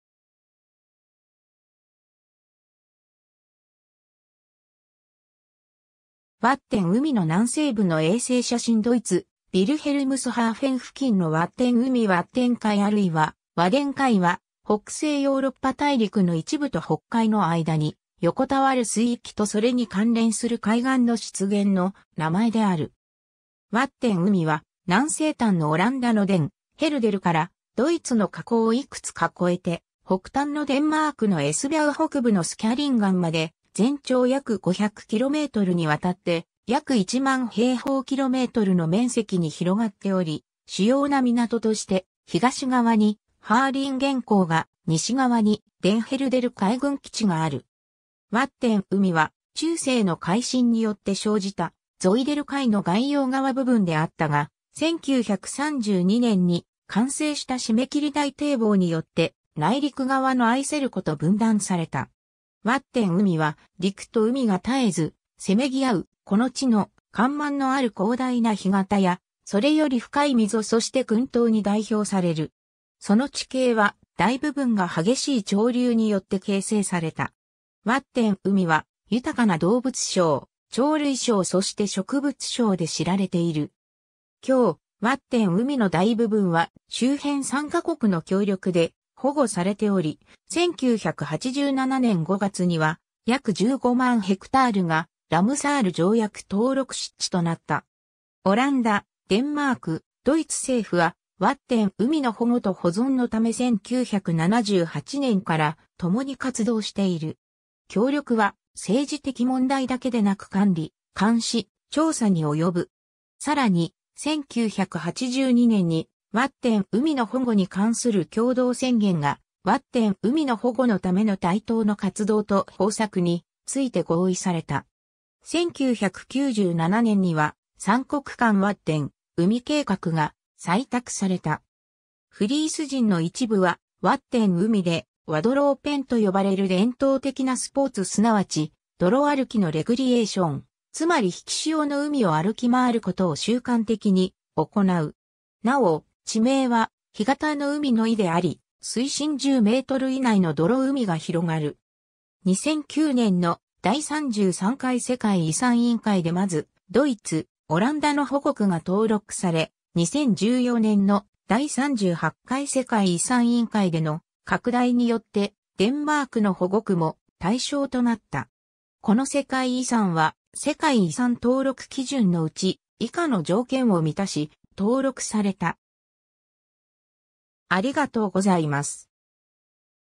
ワッデン海
ワッデン海の南西部の衛星写真 ドイツ、ヴィルヘルムスハーフェン付近のワッデン海 ワッデン海（ワッデンかい、オランダ語: Waddenzee、フリジア語: Waadsee、低地ザクセン語: Wattensee、ドイツ語: Wattenmeer、デンマーク語: Vadehavet、英語: Wadden Sea）あるいはワデン海（ワデンかい）は、北西ヨーロッパ大陸の一部と北海の間に横たわる水域とそれに関連する海岸の湿原の名前である。
 ワッデン海は、南西端のオランダのデン・ヘルデルから、ドイツの河口をいくつか越えて、北端のデンマークのエスビャウ北部のSkallingenまで、全長約 500 km にわたって、約 10,000 km2 の面積に広がっており、主要な港として東側にハーリンゲン港が、西側にデンヘルデル海軍基地がある。
 ワッデン海は、中世の海進によって生じたゾイデル海の外洋（北）側部分であったが、1932年に完成した締め切り大堤防によって内陸（南）側のアイセル湖と分断された。
 ワッデン海は、陸と海が絶えずせめぎあうこの地の、干満のある広大な干潟やそれより深い溝そして群島に代表される。
その地形は、大部分が激しい潮流によって形成された。
 ワッデン海は、豊かな動物相、鳥類相そして植物相で知られている。
今日、ワッデン海の大部分は、周辺3か国の協力で保護されており、1987年5月には約 150,000 ha がラムサール条約登録湿地となった。
 オランダ、デンマーク、ドイツ政府は、ワッデン海の保護と保存のため1978年から共に活動している。
協力は、政治的問題だけでなく管理、監視、調査に及ぶ。
さらに、1982年に、「ワッデン海の保護に関する共同宣言」が、ワッデン海の保護のための対等の活動と方策について合意された。
1997年には、「三国間ワッデン海計画」が、採択された。
 フリース人の一部は、ワッデン海でワドローペンと呼ばれる伝統的なスポーツすなわち泥歩きのレクリエーション、つまり引き潮の海を歩き回ることを習慣的に行う。
 なお、地名は「干潟の海」の意であり、水深 10 m 以内の泥海が広がる。
 2009年の第33回世界遺産委員会でまずドイツ、オランダの保護区（長さ約 400 km、面積約 10,000 km2 にわたる海岸線一帯）が登録され、2014年の第38回世界遺産委員会での拡大によって、デンマークの保護区も対象となった。